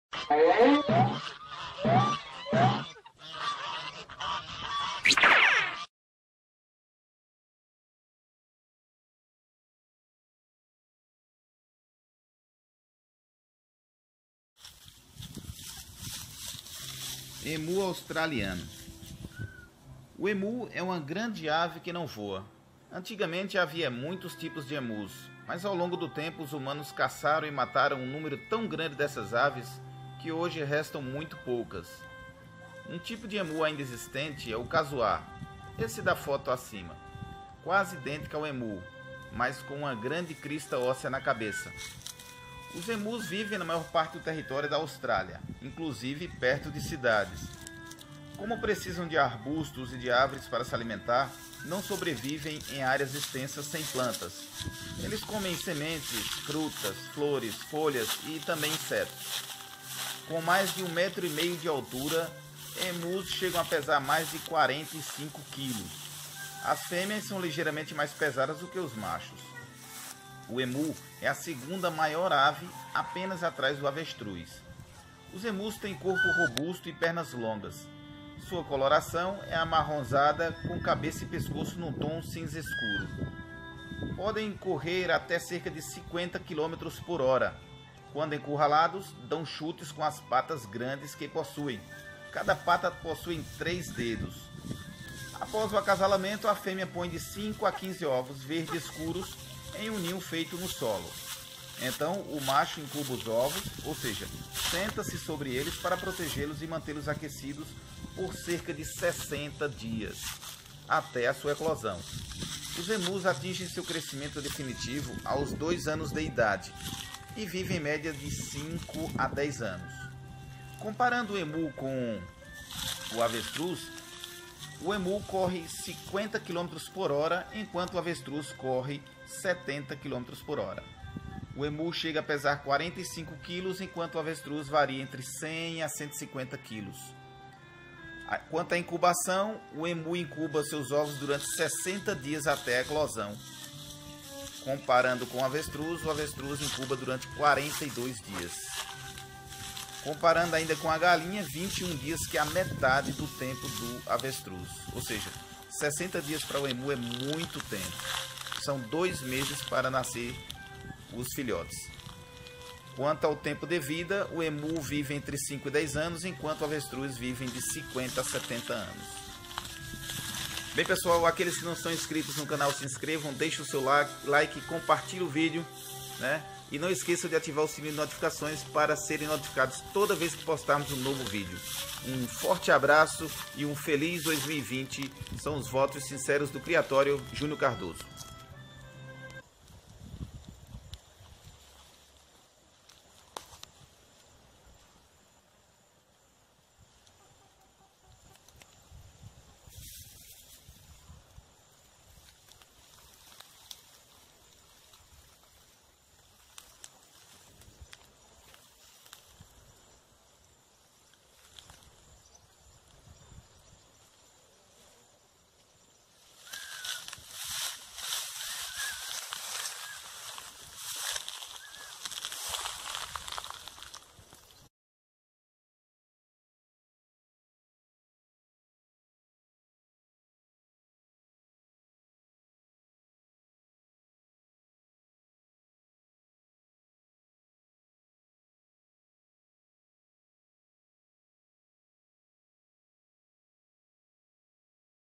EMU AUSTRALIANO, CARACTERÍSTICAS E CURIOSIDADES - SEMELHANÇA DO EMU COM O CASUAR E O AVESTRUZ
EMU

O emu é uma grande ave que não voa. Antigamente havia muitos tipos de emus, mas ao longo do tempo os humanos caçaram e mataram um número tão grande dessas aves que hoje restam muito poucas. Um tipo de emu ainda existente é o casuar, quase idêntico ao emu, mas com uma grande crista óssea na cabeça.

Os emus vivem na maior parte do território da Austrália, inclusive perto de cidades. Como precisam de arbustos e de árvores para se alimentar, não sobrevivem em áreas extensas sem plantas. Eles comem sementes, frutas, flores, folhas e insetos.

Com mais de 1,5 metro de altura, emus chegam a pesar mais de 45 quilos. As fêmeas são ligeiramente mais pesadas do que os machos. O emu é a segunda maior ave, atrás apenas do avestruz. Os emus têm corpo robusto e pernas longas. Sua coloração é amarronzada, com cabeça e pescoço no tom cinza-escuro. Podem correr até cerca de 50 quilômetros por hora; quando encurralados, dão chutes com as patas grandes. Cada pata tem três dedos.

Após o acasalamento, a fêmea põe de cinco a quinze ovos verde-escuros em um ninho feito no solo. Então o macho incuba os ovos, ou seja, senta-se sobre eles para protegê-los e mantê-los aquecidos por cerca de 60 dias, até sua eclosão. Os emus atingem seu crescimento definitivo aos 2 anos de idade e vivem, em média, de 5 a 10 anos.

Algumas de nossas aves:
Aves e ovos galados a venda.

Contatos:
Whatszaps Tim (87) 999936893 e Vivo (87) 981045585.